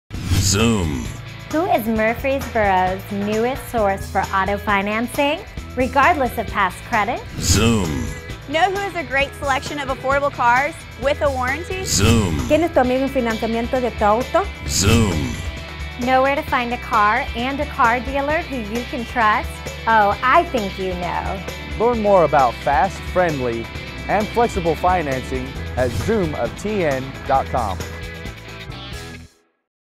Meggan Conti Zoom Auto Finance Local Commercial - Murfreesboro, TN
This is a local commercial I was in for Zoom Auto Finance in Murfreesboro, TN! I think you will all enjoy this one! He He!